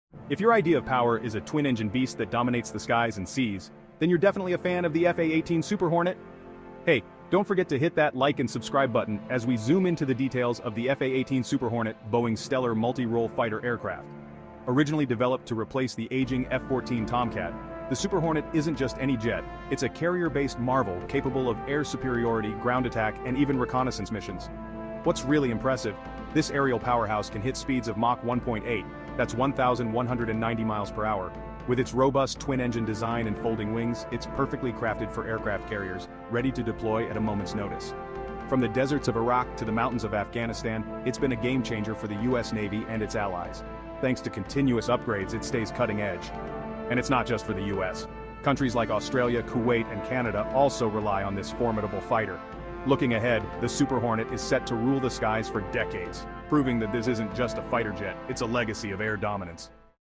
The F/A 18 Super Hornet - How Long Will it Remain in Service?
Discover the unparalleled capabilities of the F/A-18 Super Hornet, Boeing's advanced carrier-based fighter and attack aircraft. Learn about its agility, combat prowess, and versatility in air-to-air and air-to-ground missions. From its twin-engine design to cutting-edge avionics and weapon systems, explore why the Super Hornet remains a cornerstone of modern naval aviation.

An AI generated video.